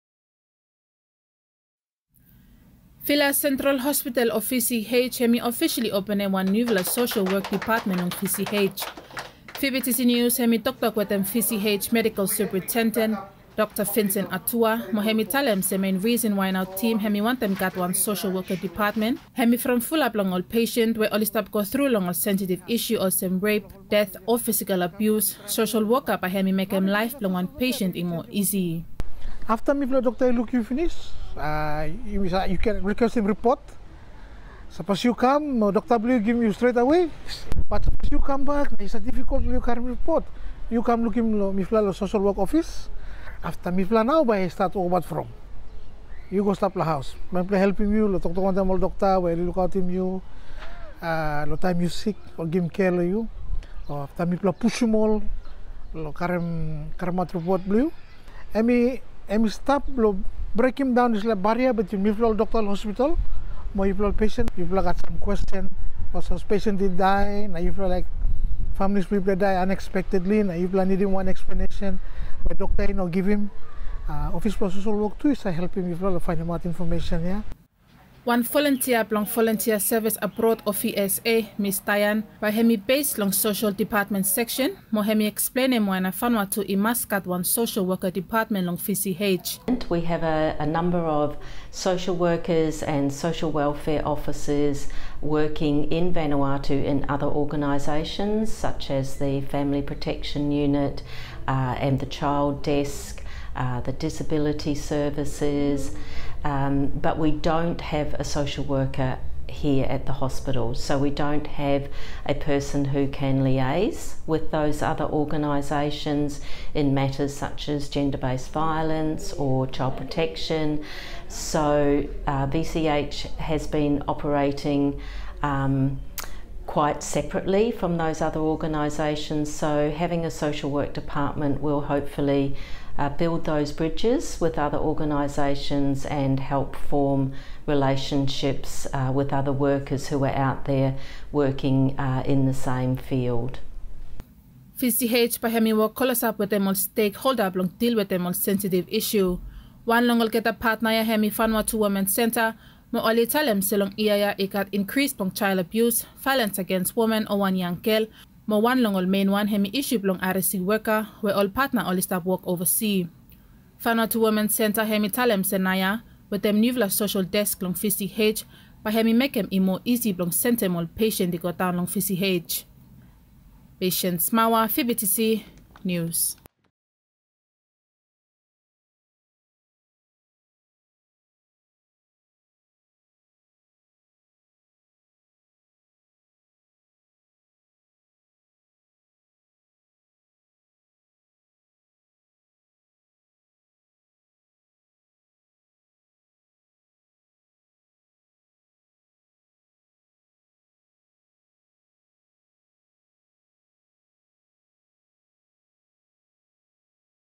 New social work department will enhances Patient-Doctor communication | VBTC News
The opening of the new social work department at Vila Central Hospital (VCH) aims to help enhance Patient-Doctor communication more easily and effectively.

Upon request for families about medical condition of patients or seeking doctors and nurses for help, the social worker department will provide more assistance according to VCH.